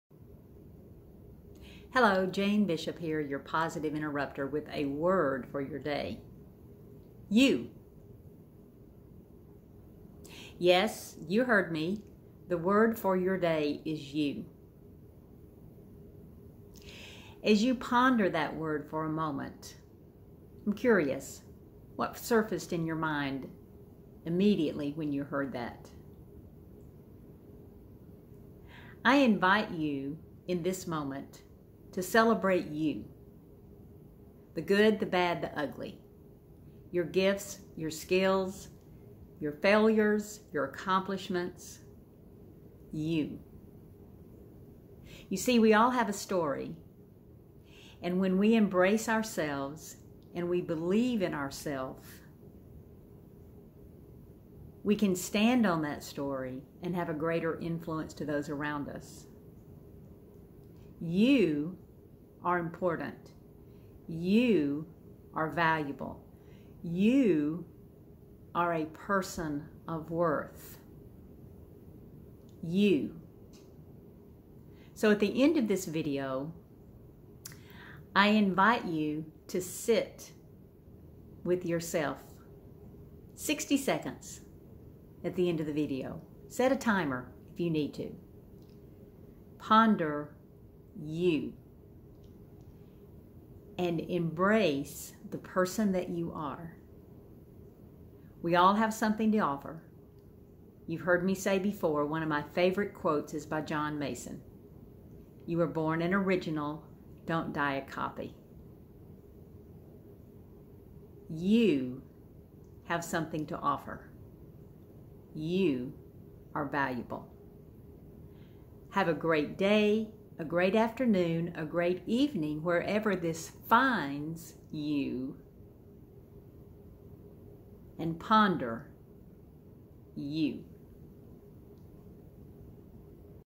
A word for your day...you!
What comes to mind when asked to ponder "you?" Take time to embrace YOU and stand on your story.